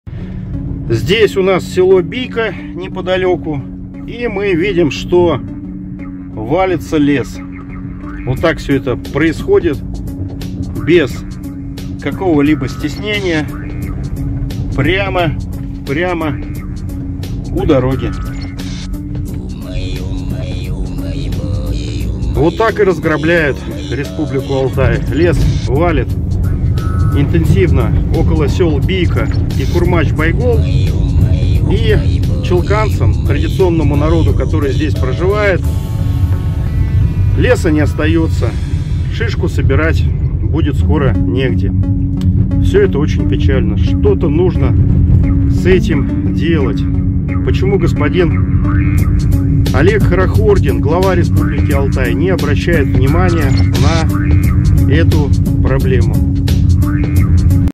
Республика Алтай! Рубят лес...😪 Бийка, Курмач-Байгол, Турочак, алтайцы, челканцы
В Республике Алтай вырубают лес возле сёл Бийка и Курмач-Байгол...🔥😪 Специально сьезил, чтобы посмотреть на это...  Рубят не стесняясь даже около дороги...😪
Прошу Главу Республики Алтай Олега Хорохордина обратить внимание на эту вырубку. Прошу Прокуратуру Республики Алтай провести проверку по данному факту на предмет законности.